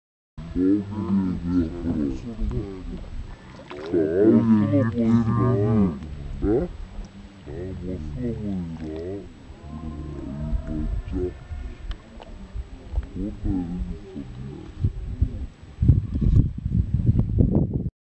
Near the water, Munnar Echo point, Kerala | Full HD 1080p
This scenic place gets its name from the natural echo phenomenon here. Echo Point, situated on the way to Top Station from Munnar is a stop over for tourists visiting Top Station - the highest point in Munnar and the rare Neelakurinji (Strobilanthus) blooms here.